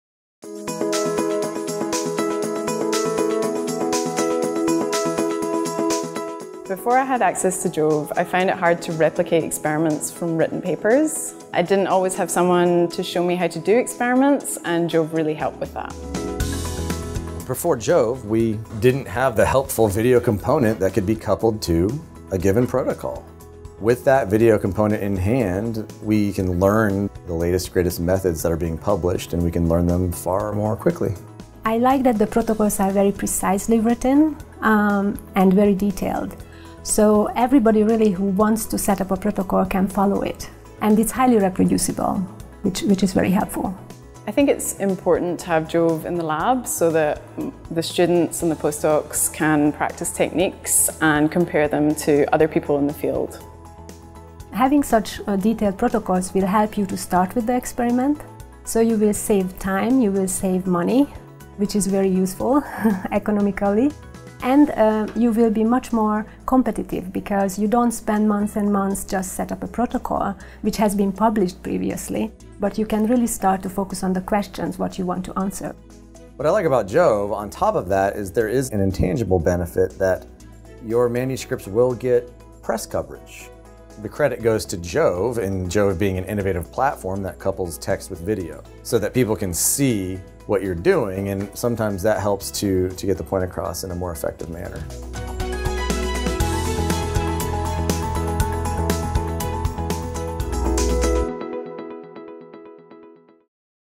Benefits of Incorporating JoVE in the Research Lab
Scientists and researchers are learning and publishing with JoVE Video Journal to set their labs up for success.